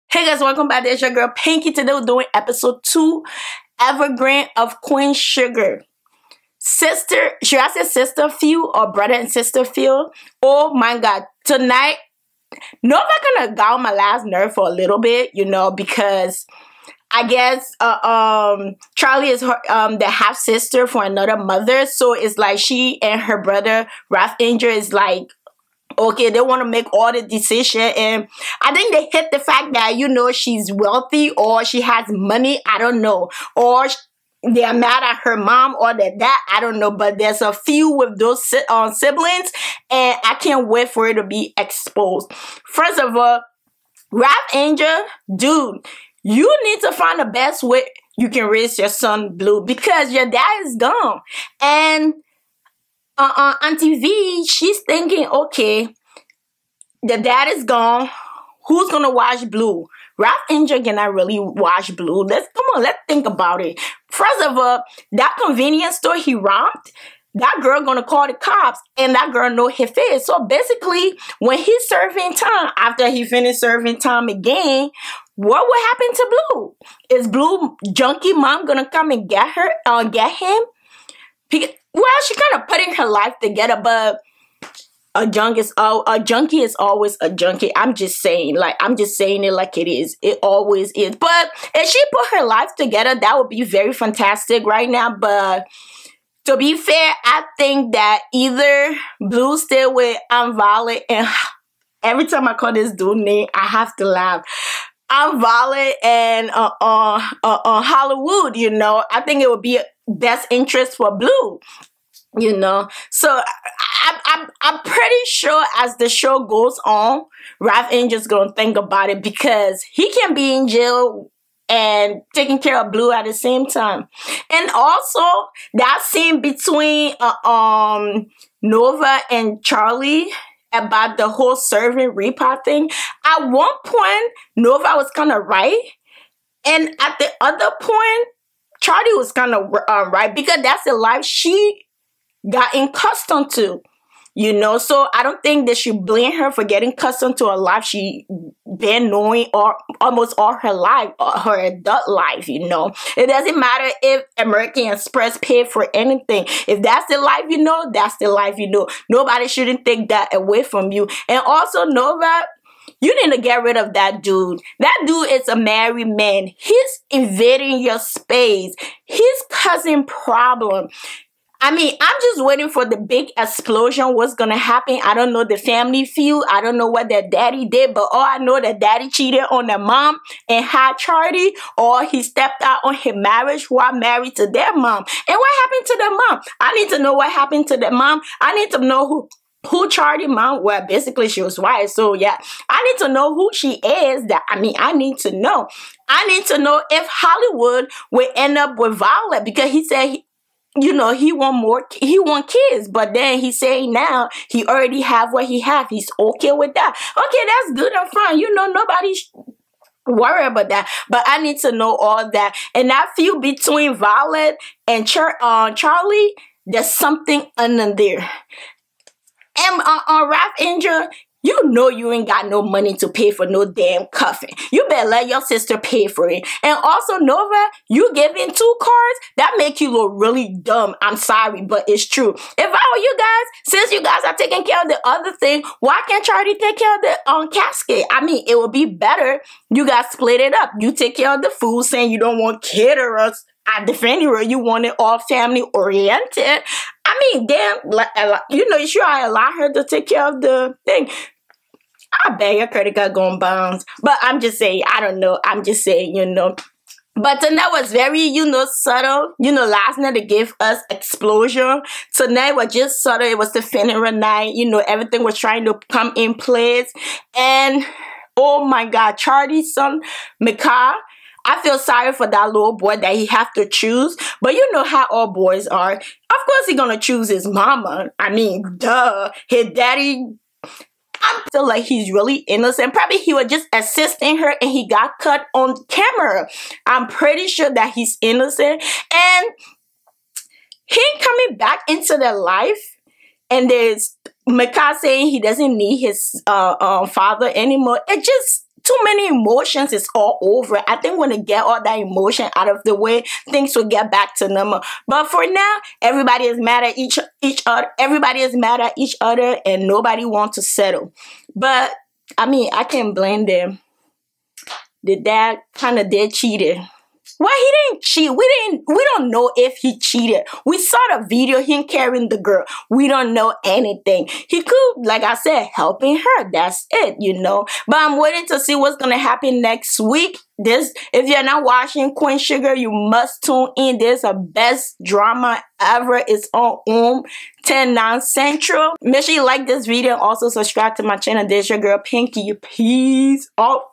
(Review) Queen Sugar | Rutina Wesley vs Dawn-Lyen Gardner | Season 1: Ep. 2 | Evergreen (Recap)
Queen Sugar Eps 2 Planning of their Father Funeral but unfortunately Nova wasn't taking it easy with Charley their half sister. How will these siblings do now their father is dead. Queen Sugar is a must seen Tv drama on own network